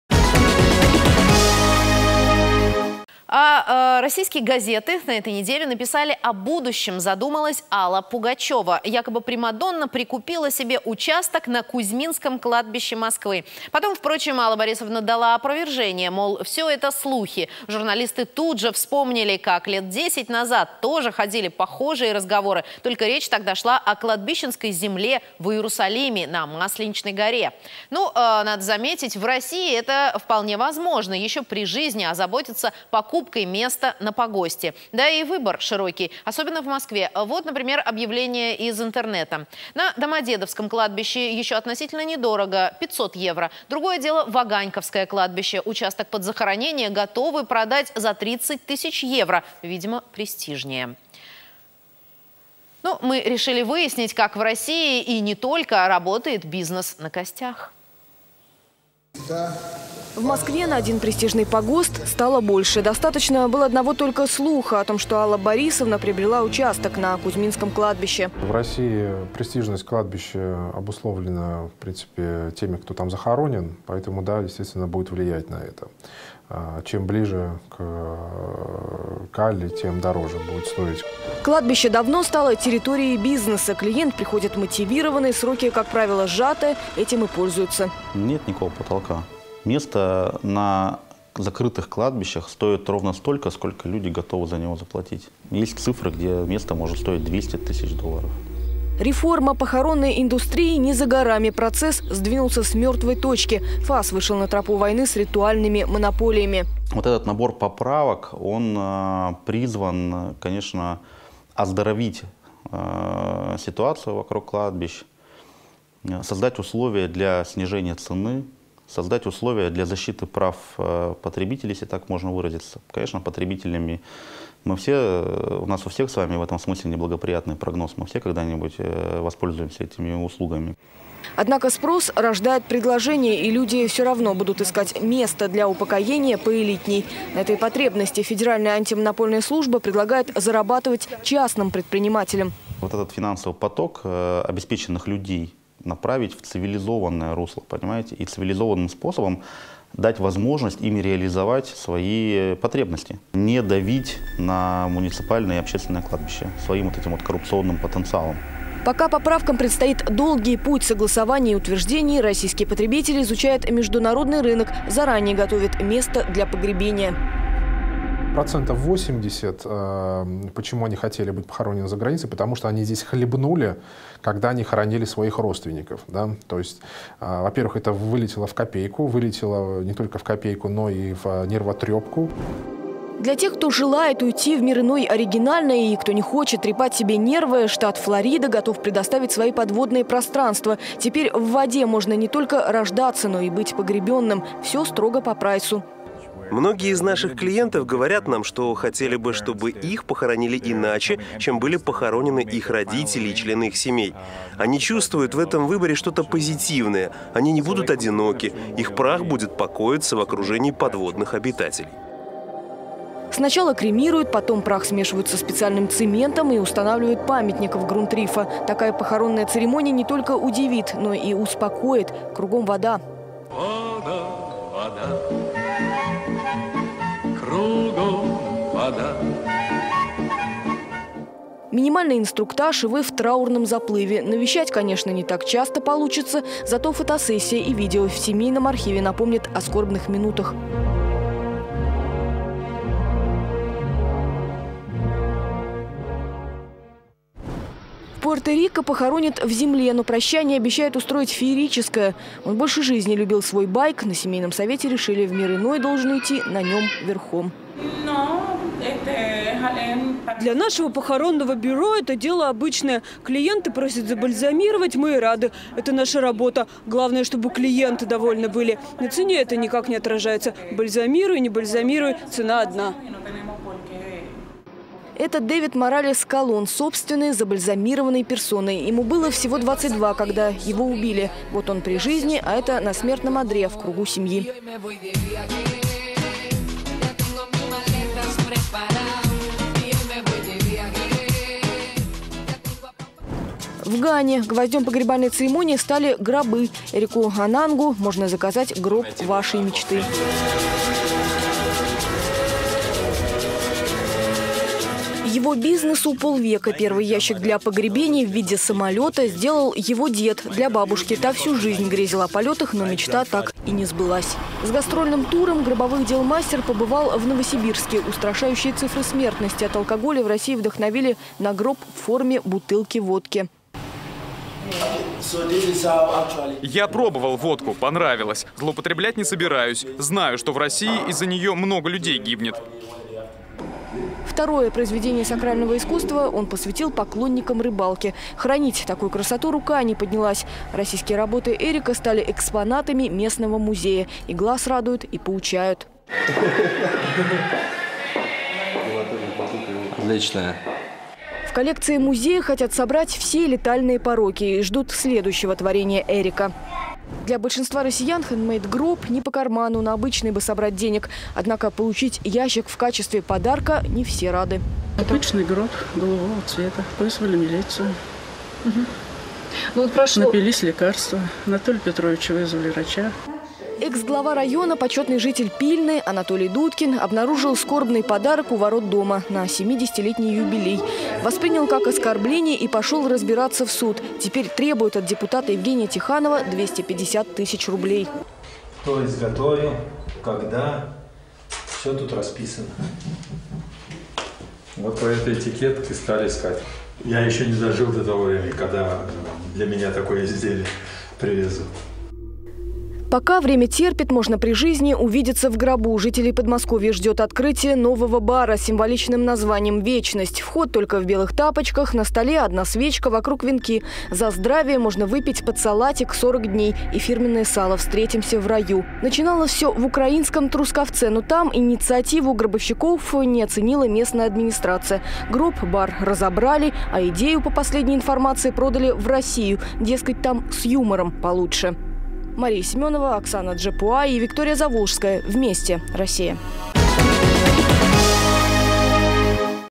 Купить место на кладбище. Эфир 9.09.2012
Российские газеты на этой неделе написали - "О будущем задумалась Алла Пугачева". Якобы Примадонна прикупила себе участок на Кузьминском кладбище Москвы. Потом, впрочем, Алла Борисовна дала опровержение. Мол, все это слухи. Журналисты тут же вспомнили - как лет десять назад тоже ходили похожие разговоры. Только речь шла о кладбищенской земле в Иерусалиме - Масличной горе.

Ну, надо заметить в России это вполне возможно - еще при жизни озаботиться покупкой места на погосте. Да и выбор - широкий. Особенно, в Москве. Вот, например, объявления из интернета